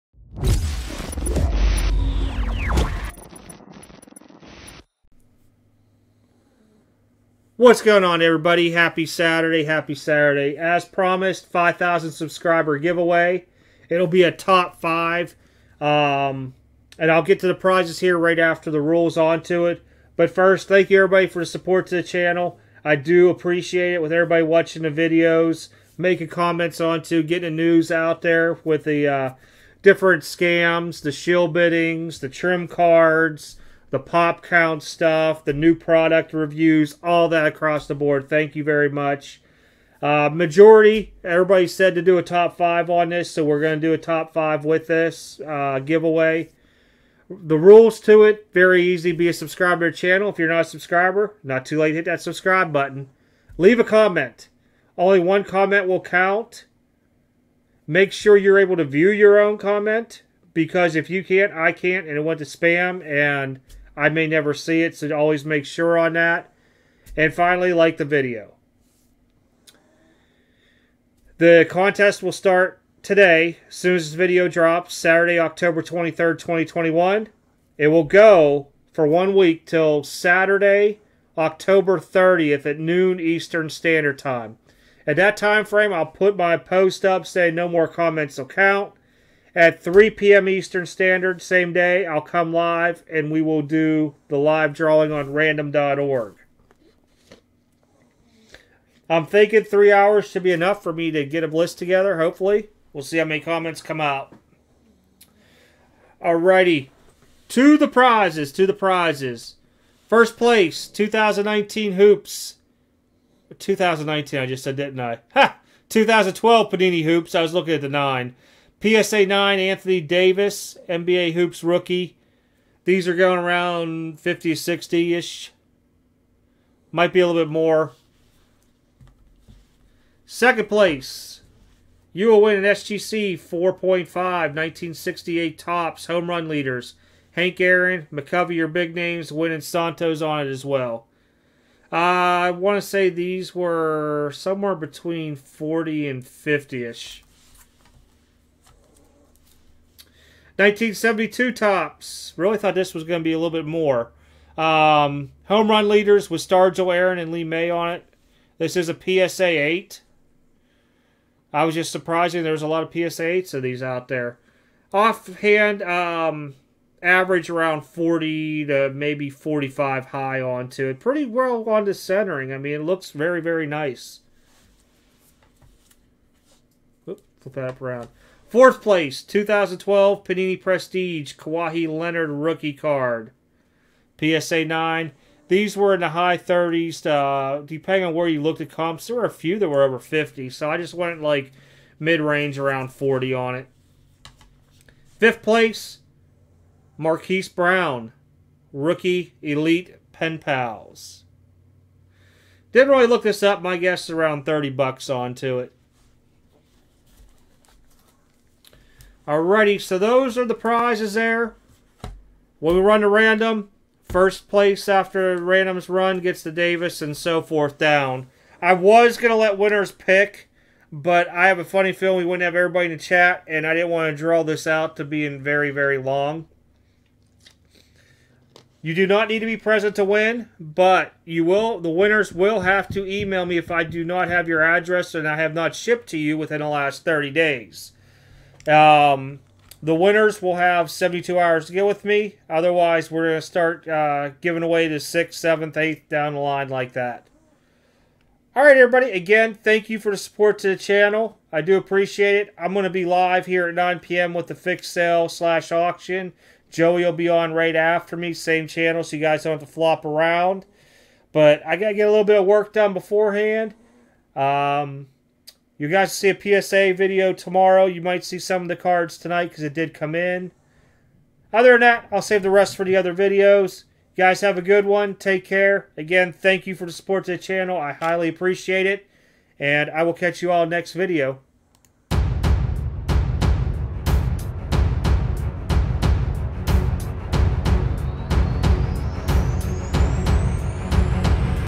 Entry Video - 5000 Subscriber Giveaway - Rules also in Description
Rules:
Start Time - Saturday October 23, 2021 @ Release of Video
Stop Time - Saturday October 30, 2021 @ 12PM EST
Live Random:  Saturday October 30, 2021 @ 3PM EST

3.  Like the Video

Live Random will be done via Random.org using my verified account.  The cards in the video are stated to what position they will go to with the final round of the random.  If you fail to do that, I move down the list.

1811 N Dixie Ave #104

Who do I collect:
Roberto Clemente
Michael Jordan
Nolan Ryan
Sidney Crosby
Vintage Cards